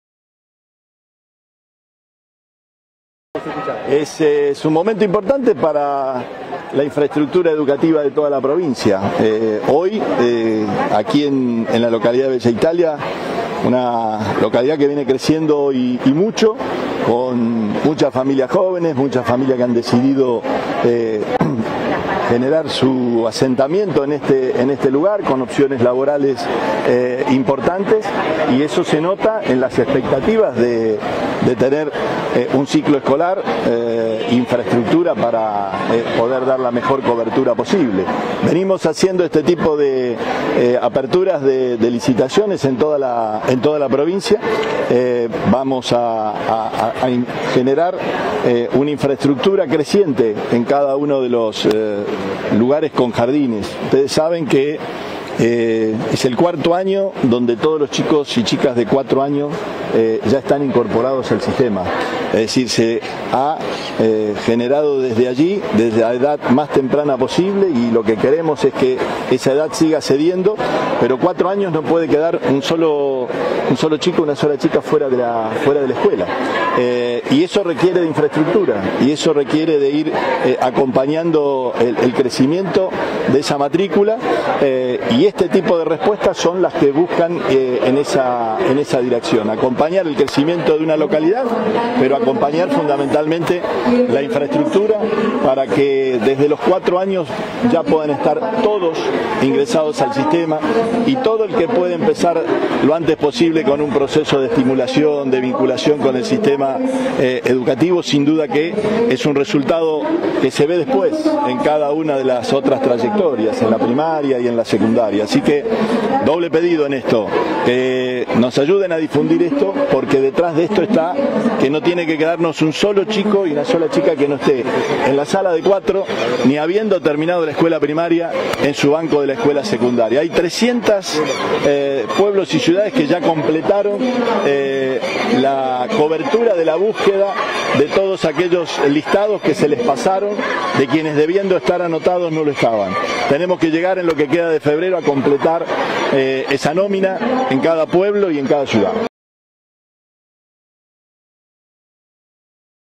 PEROTTI PARTICIPÓ DE LA LICITACIÓN PARA LA CONSTRUCCIÓN DE UN JARDÍN DE INFANTES EN BELLA ITALIA
“Desde nuestro primer día de gestión, la iniciativa fue que todos nuestros chicos y chicas, estén incorporados al sistema educativo con la mejor infraestructura posible”, afirmó el gobernador durante el acto.